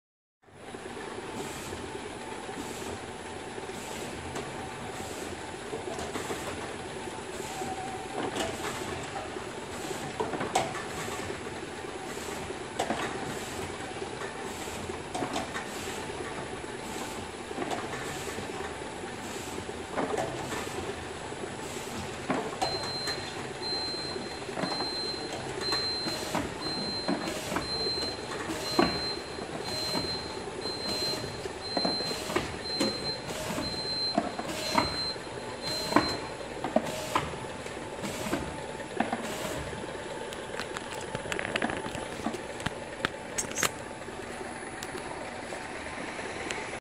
Apex Filling Systems- Zero Down Time Automatic Wrap Labeler
Model PRO515ZDT Automatic Wrap Labeler with (2) label spools. When labels run out on the first spool, this machine will automatically change over to the second spool and continue labeling your containers. The labeler will alert the operator of an empty label roll, and allow the roll to be replaced without shutting the machine down.